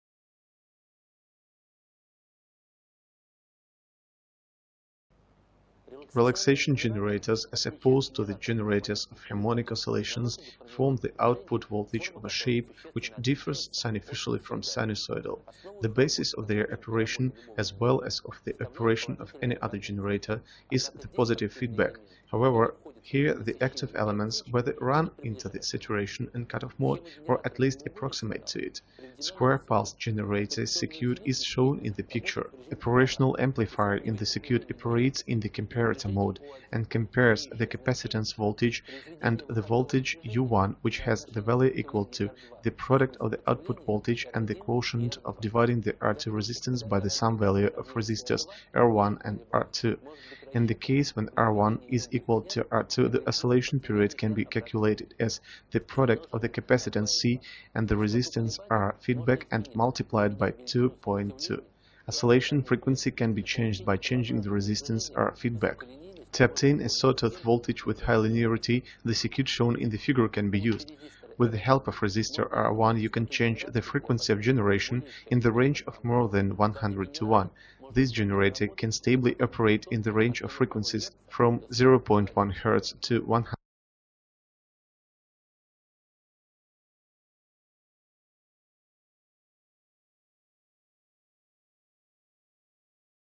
Opamp relaxation oscillator
Relaxation generators, as apposed to the generators of harmonic oscillations, form the output voltage of a shape which differs significantly from sinusoidal. The basis of their operation, as well as of the operation of any other generator, is the positive feedback.